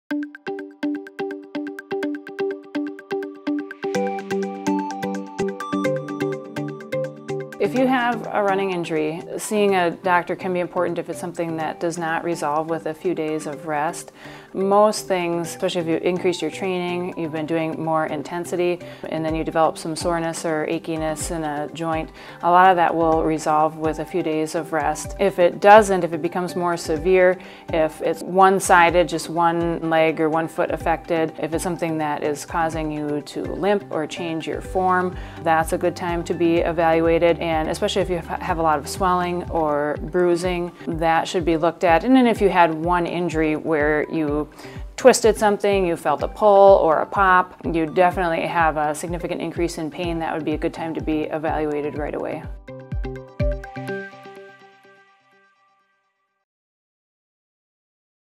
When should I see a doctor for running injury?
Listen to Summit Orthopedics' experts share some tips on when the best timing is when you are concerned about a running injury. Featuring sports medicine physician and avid runner, Angela Voight, MD. For more information or to find an expert, visit us at summitortho.com.